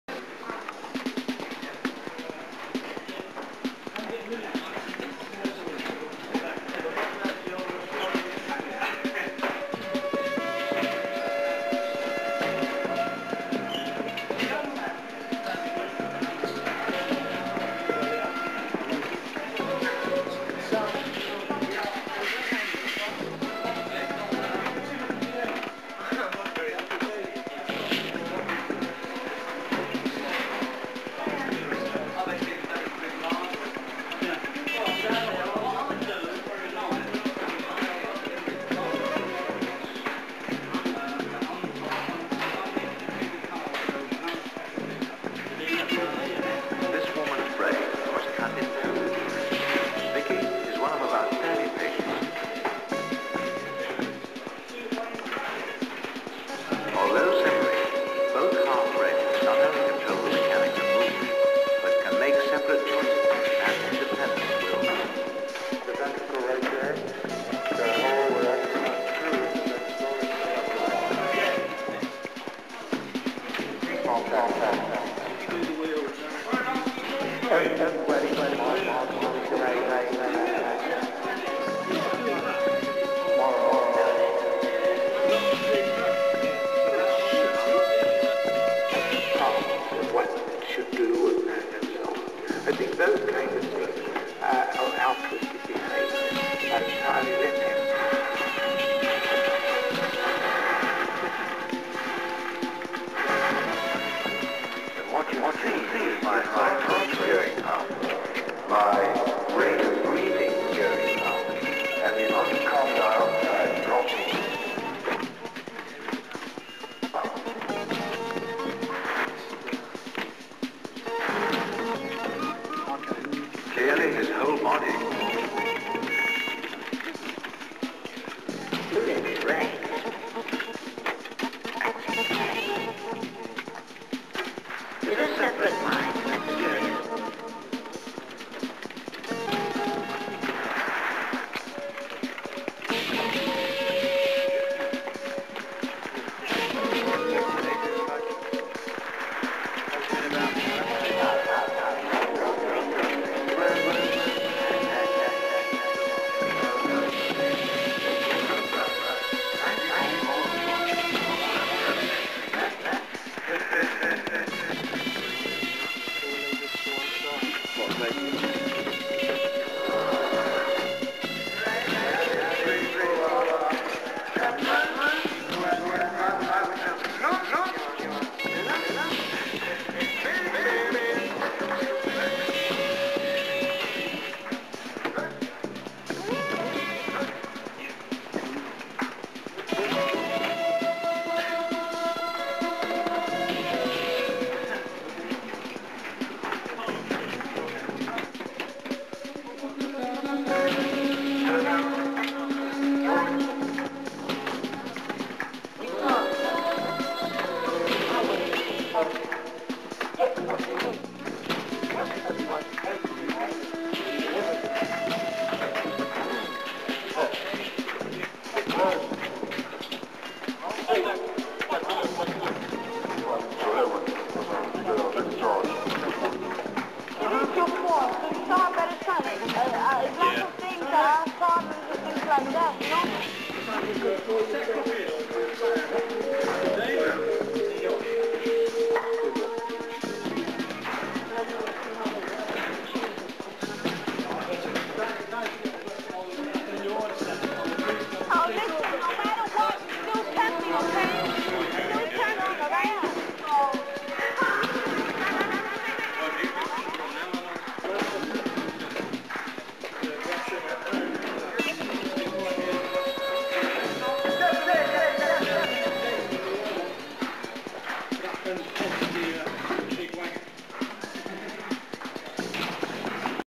Bedford Boys Club - Crowd clip 2
Filmed at the Cabs soundcheck in the afternoon of the 18th Aug 1984